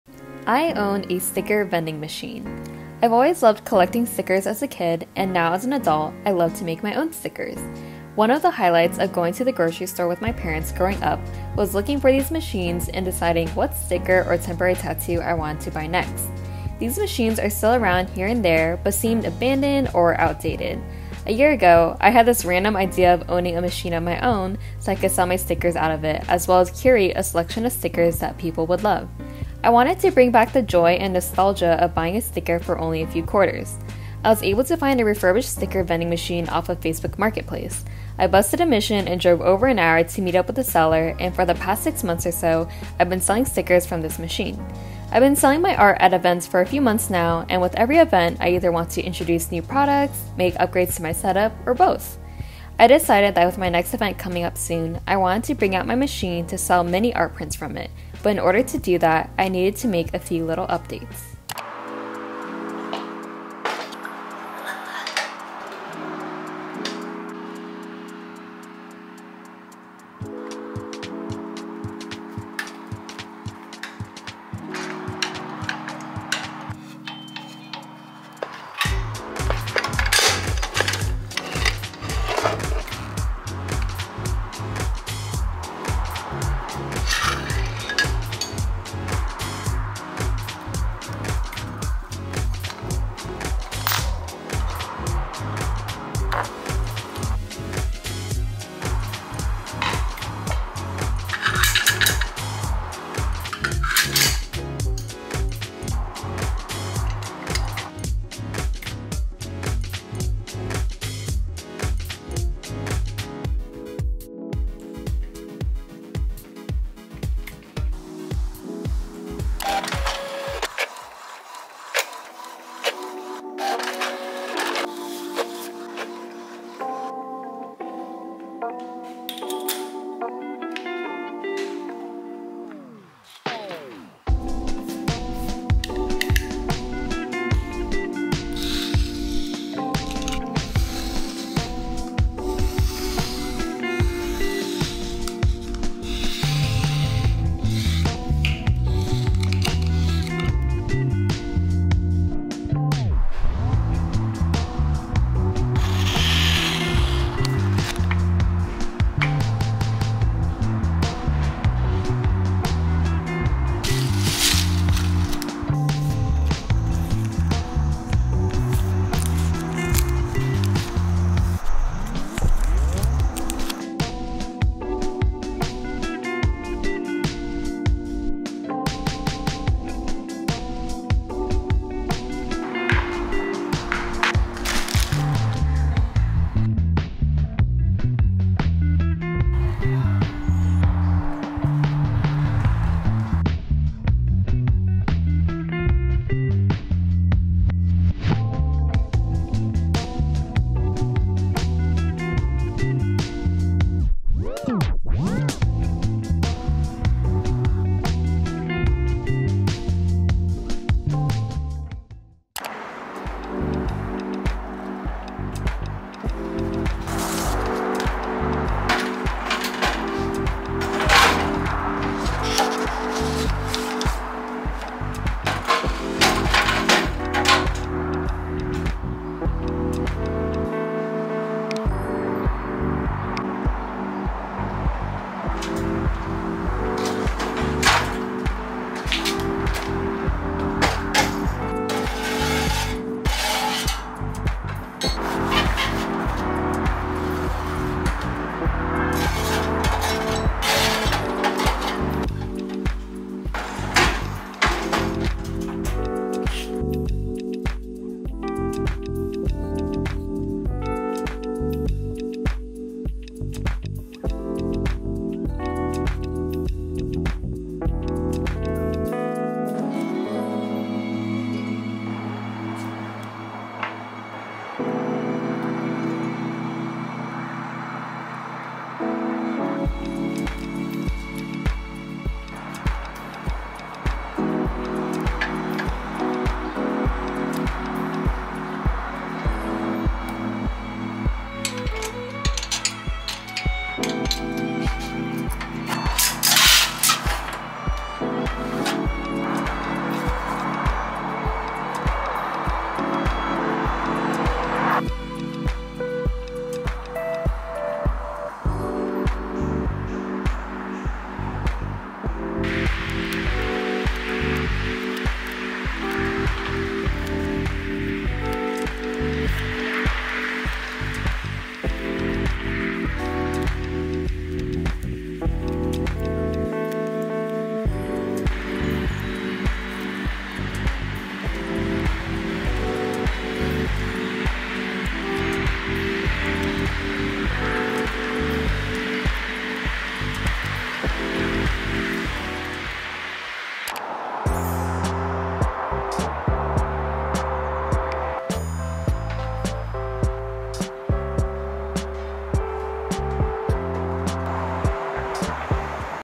how I sell my art from a sticker vending machine
The story of how I randomly bought a sticker vending machine and how I sell my art out of it

Find me at my next event!
Saturday 7/22 | 12PM-6PM
Soul Real Market @ Troy HS (Fullerton, CA)